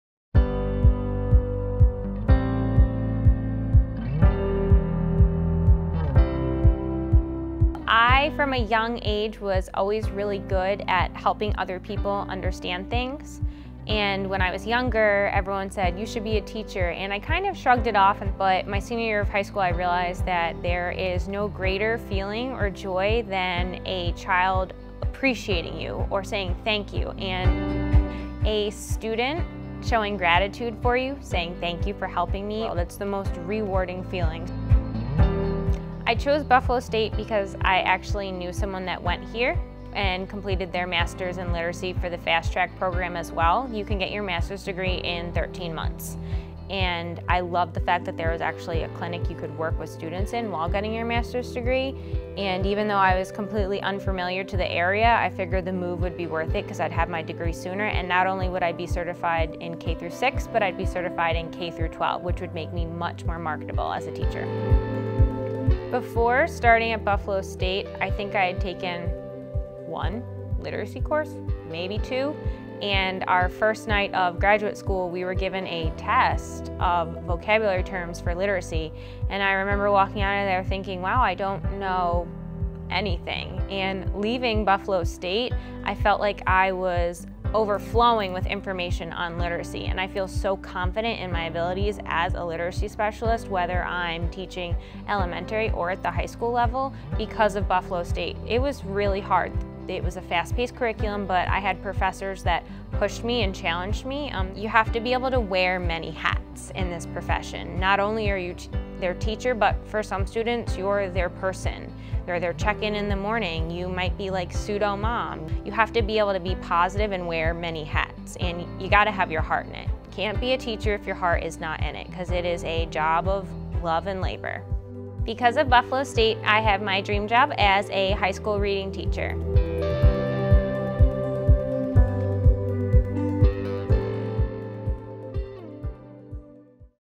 School of Education Graduate Profile: Amber Walther
Amber, graduate in the Literacy Specialist master’s program, discusses the “love and labor” of teaching, and how Buffalo State helped her land her dream job as a high school reading teacher.